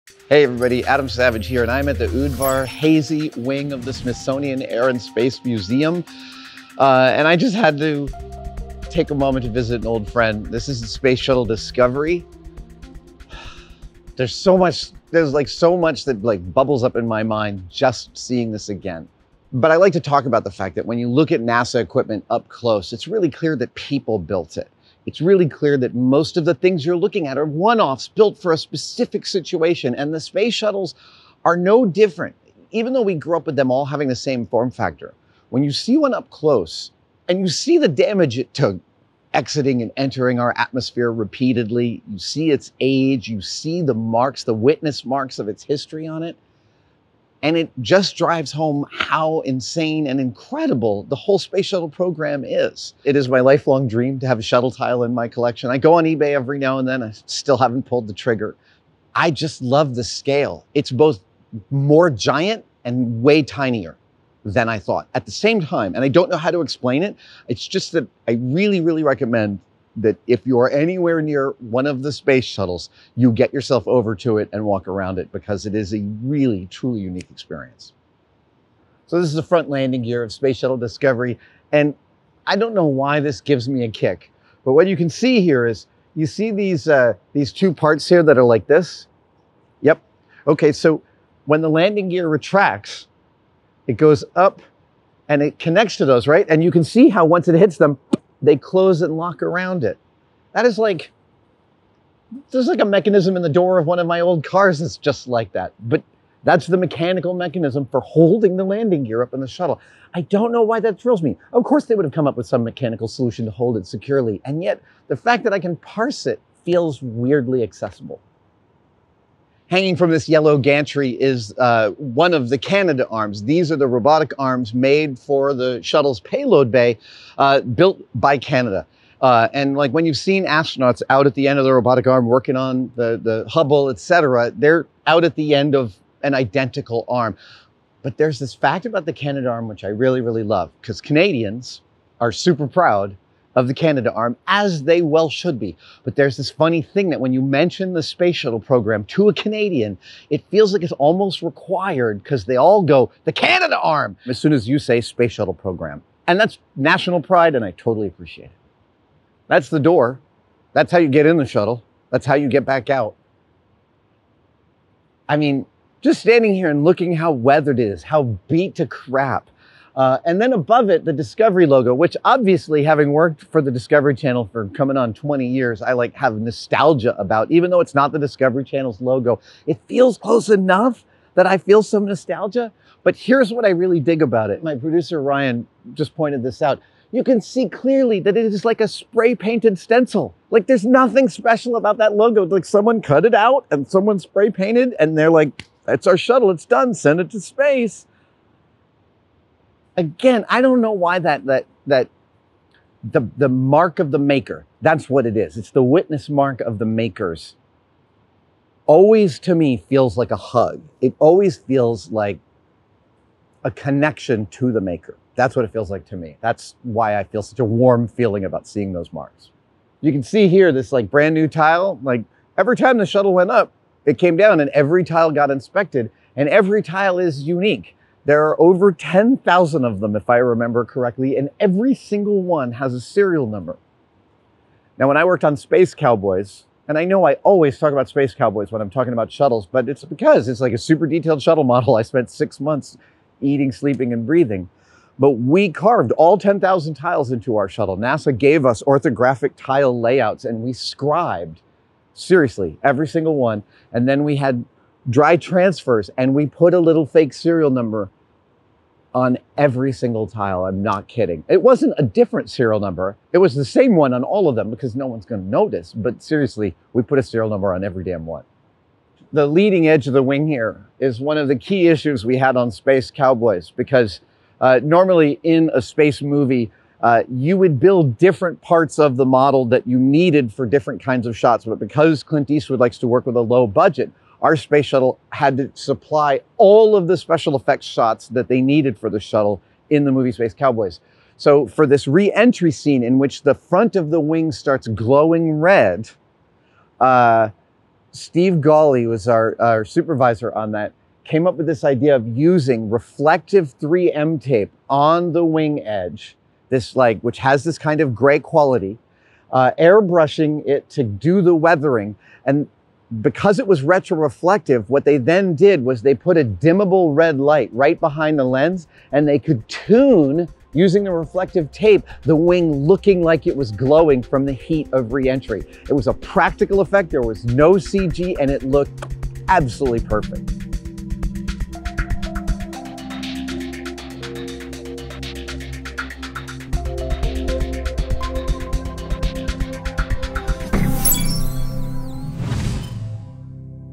Why the Space Shuttle Discovery Delights Adam Savage SO MUCH
When Adam Savage visited the National Air and Space Museum -- specifically the Udvar-Hazy Center -- in August 2021, he stopped to marvel at an old friend, the Space Shuttle Discovery. From its stenciled logo to its pattern of damaged tiles to its TINY door, Adam highlights the many features of this historic aircraft that never cease to delight him.

Produced by Kristen Lomasney